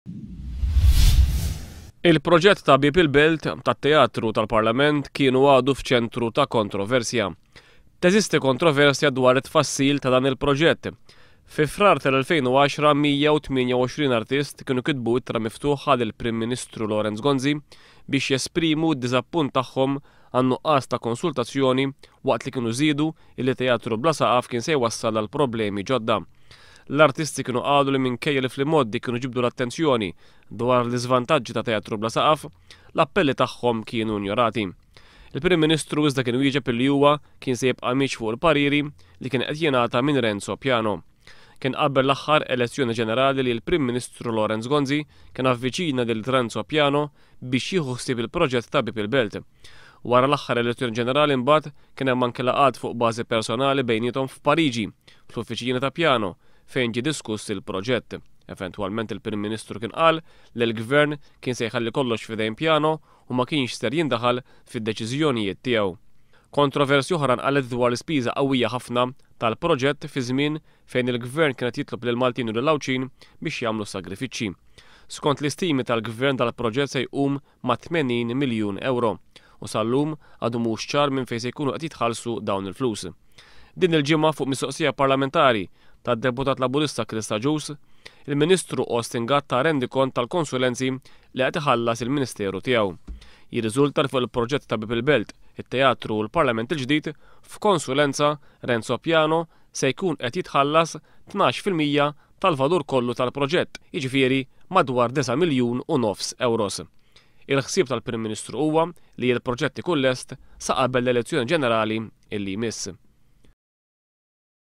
Renzo Piano qed jirċievi kważi €10,000,000 fi ħlasijiet għal konsulenzi.
Il-kumpanija ta' Renzo Piano se tkun qed tircievi kwazi ghaxar miljun ewro fi hlasijiet ghal konsulenzi fuq il-progett tal-bieb il-belt, t-teatru u l-parlament il-gdid.Dan it-taghrif inghata fil-parlament mill-Ministru Austin Gatt